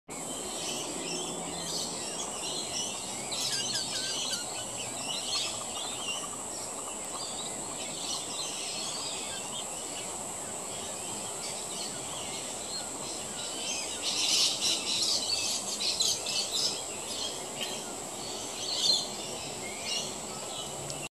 Adult and Juvenile Grey Butcherbirds
An interesting capture between an adult and juvenile grey butcherbird.